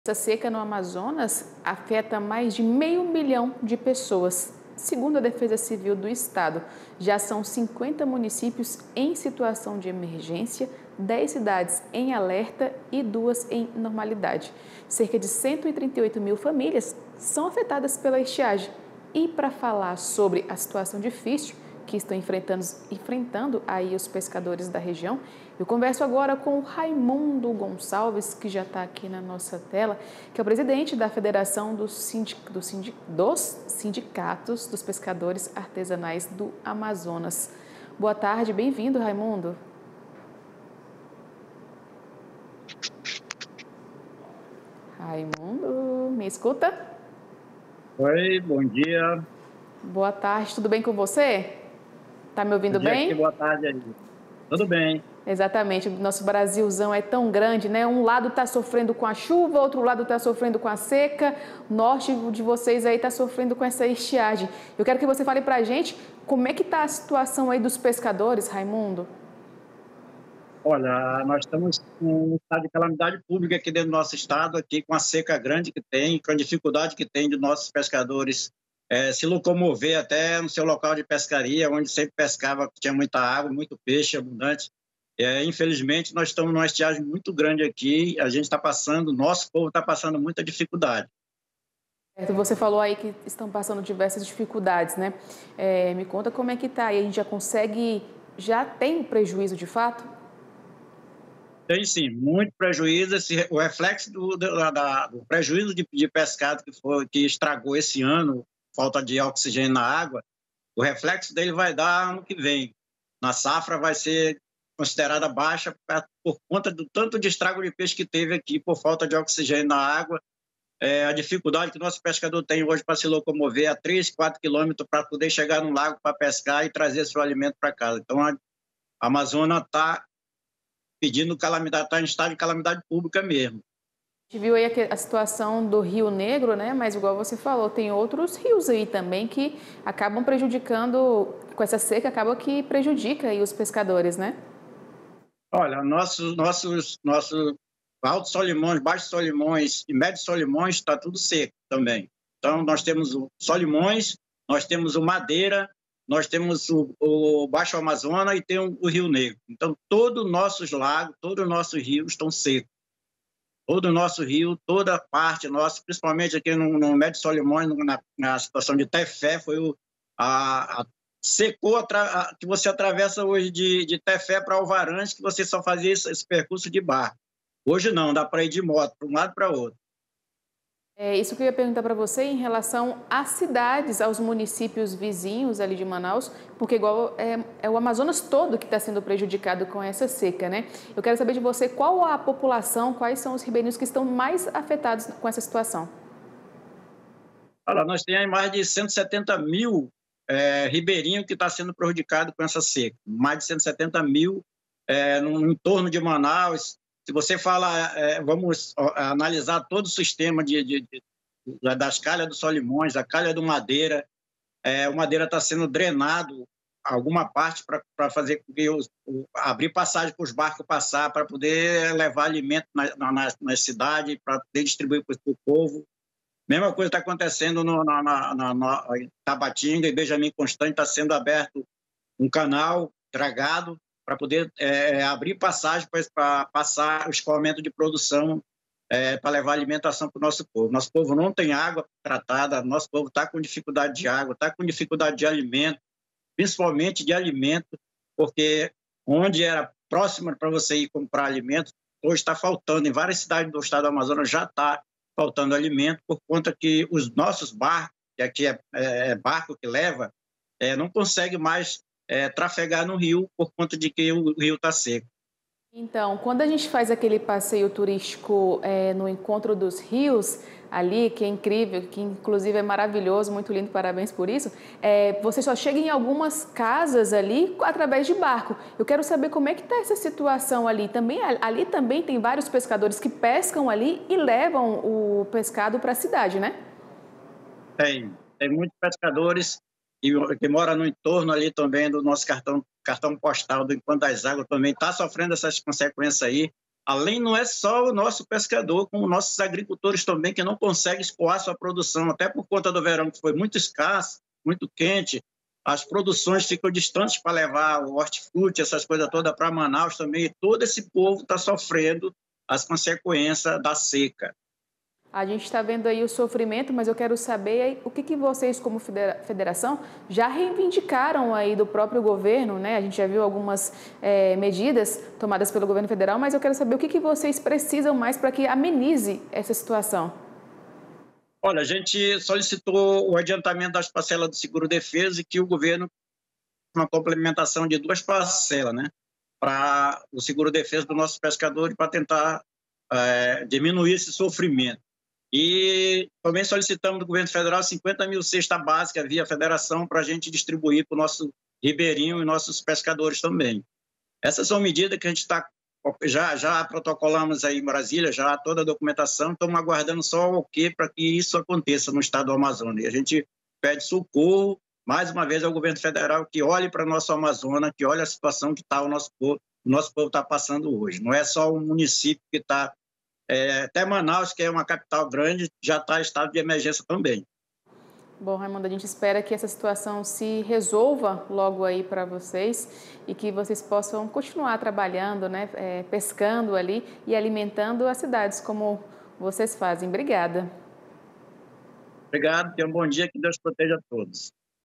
138 mil famílias são atingidas pela estiagem no Amazonas
Cerca de 138 mil famílias do Amazonas são afetadas por essa estiagem. O setor pesqueiro, é claro, é um dos que mais sofrem com essa situação. O momento é delicado aos pescadores da região e o Raimundo Gonçalves, presidente da Federação dos Sindicatos dos Pescadores e Pescadoras Artesanais do Amazonas, fala sobre isso.